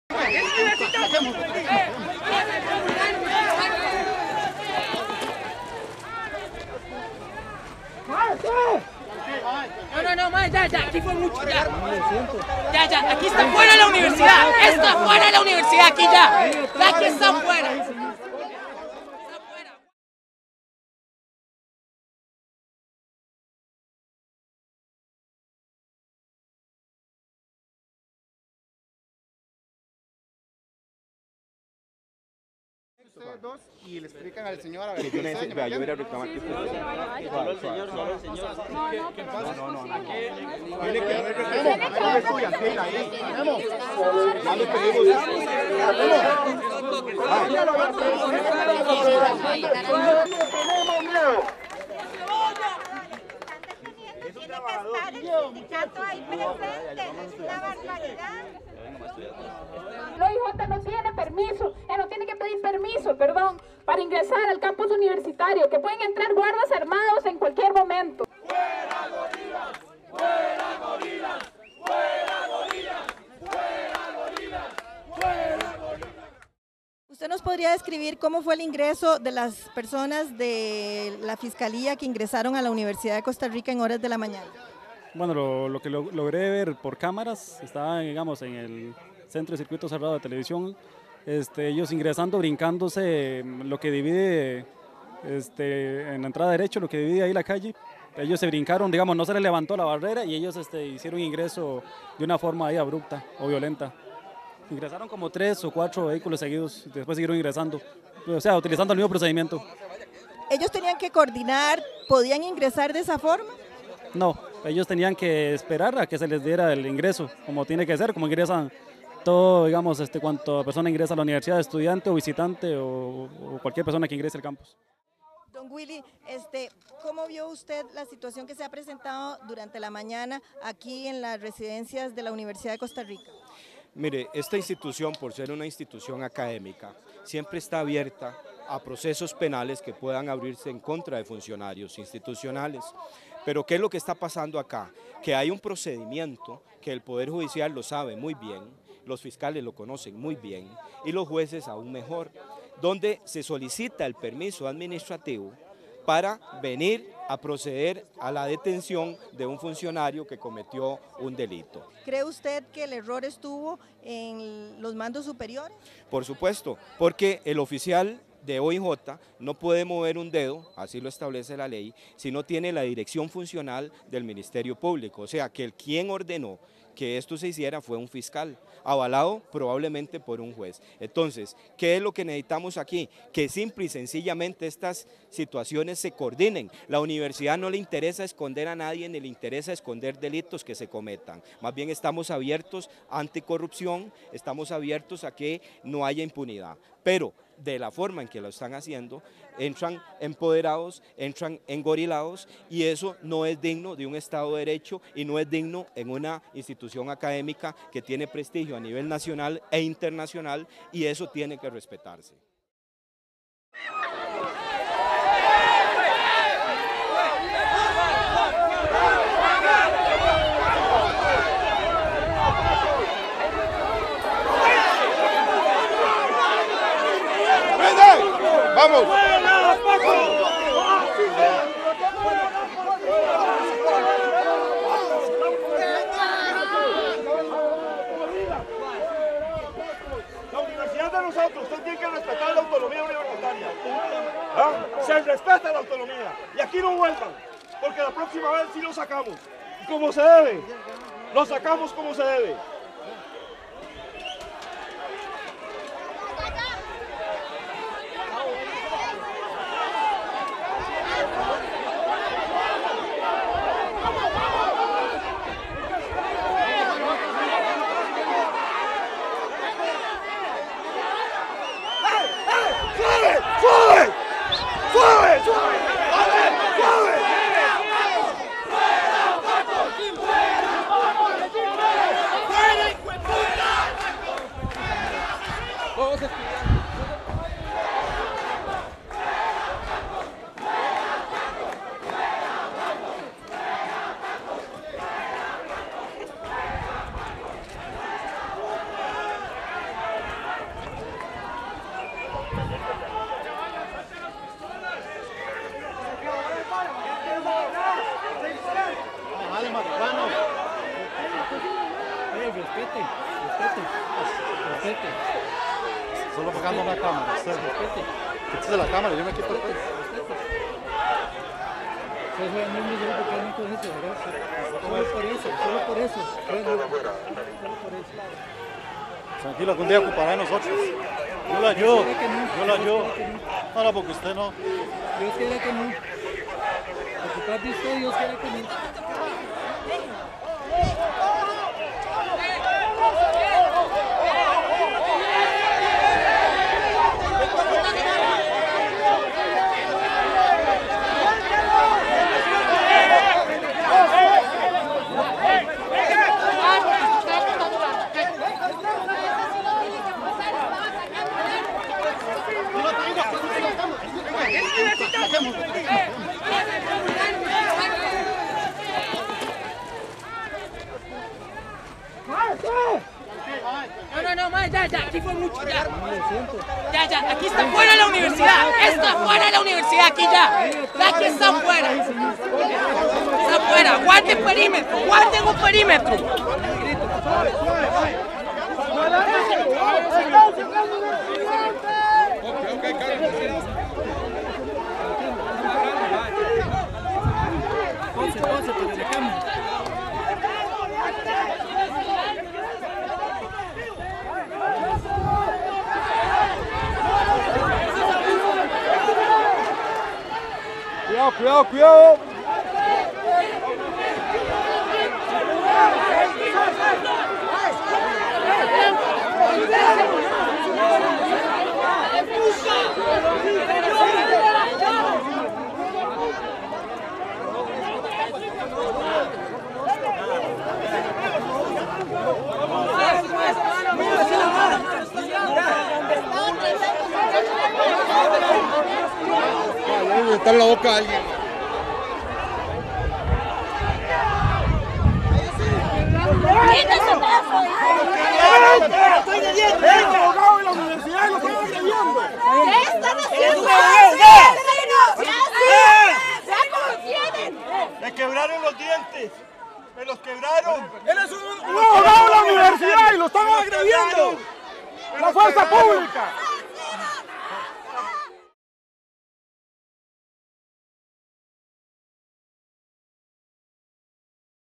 Reportaje Especial - Violencia policial en Universidad de Costa Rica
12 de abril de 2010.  Agentes oficiales del Organismo de Investigación Judicial ingresan a la Universidad de Costa Rica armados y sin haber solicitado ninguna autorización.  Ante esta violación a la autonomía universitaria, estudiantes, profesores y funcionarios de la Universidad se manifiestan, y por eso son agredidos brutalmente por las fuerzas policiales.